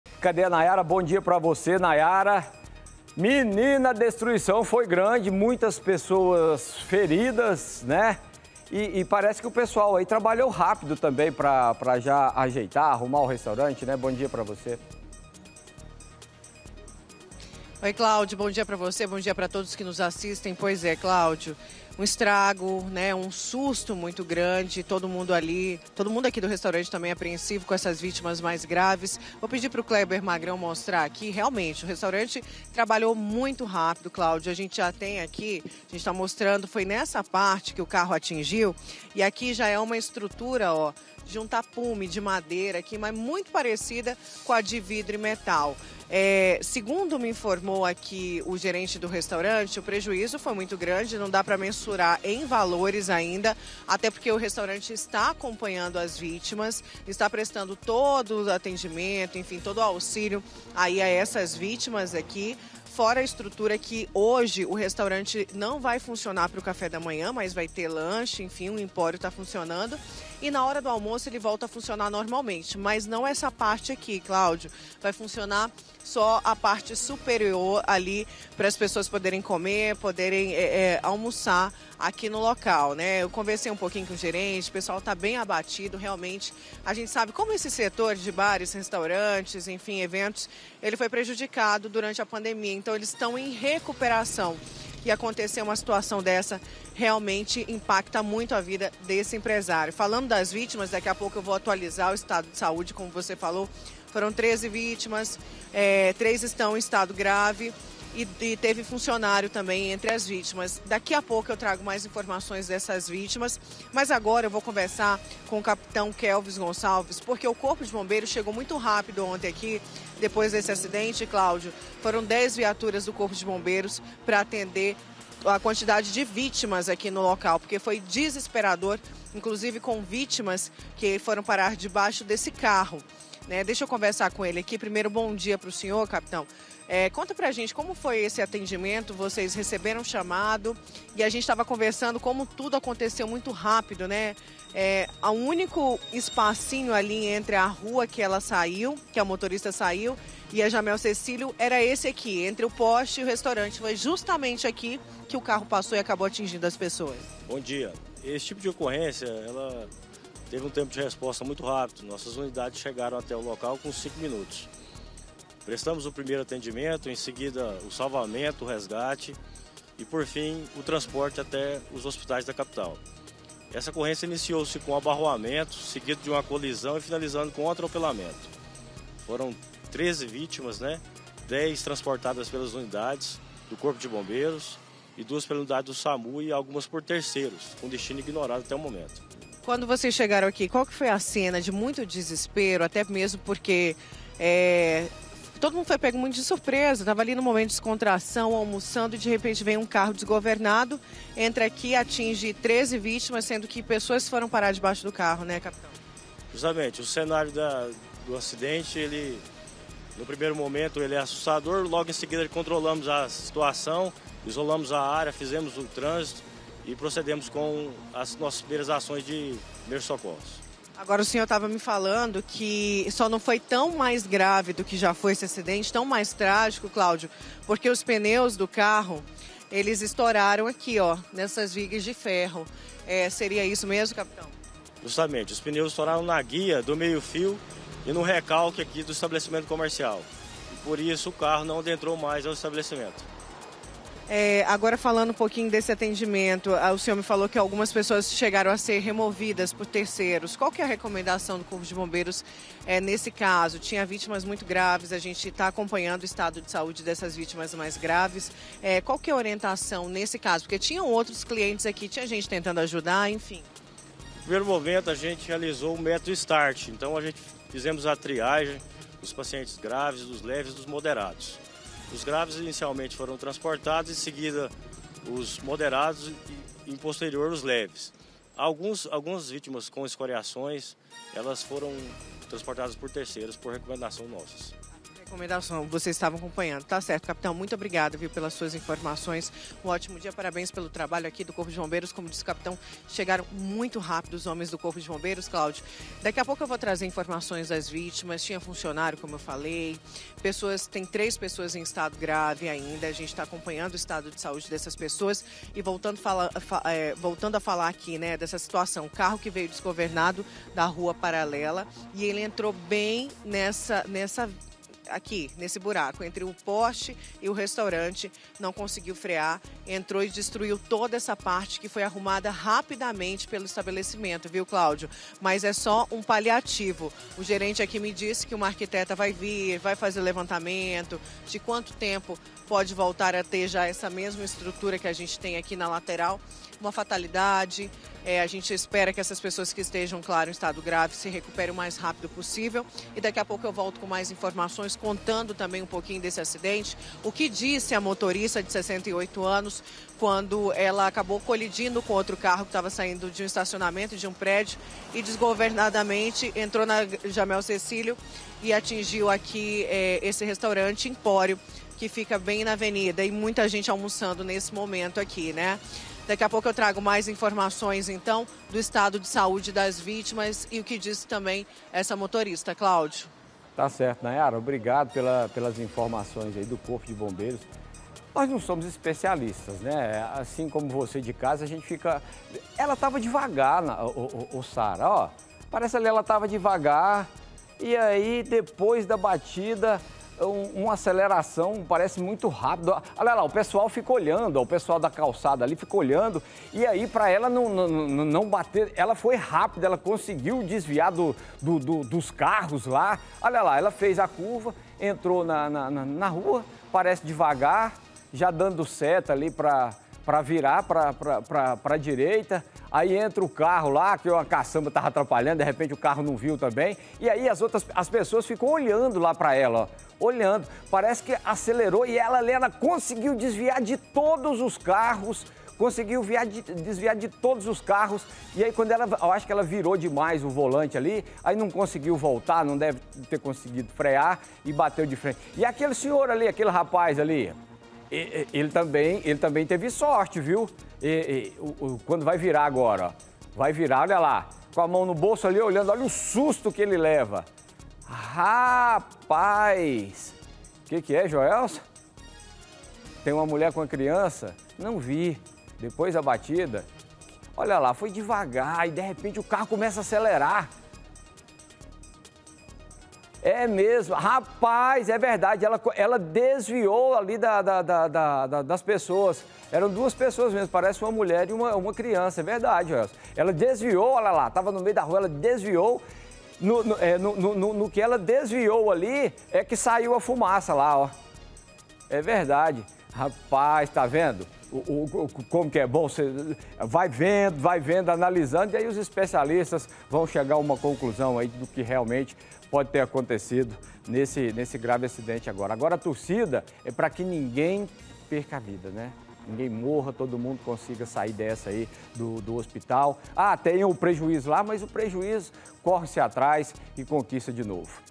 ACIDENTE EM RESTAURANTE: PERÍCIA NO CARRO SERÁ FEITA HOJE
A GENTE VIU QUE A DESTRUIÇÃO FOI GRANDE, MUITAS PESSOAS FERIDAS, O CARRO FICOU BEM ESTRAGADO COMO ESTÁ O RESTAURANTE DEPOIS DO QUE HOUVE?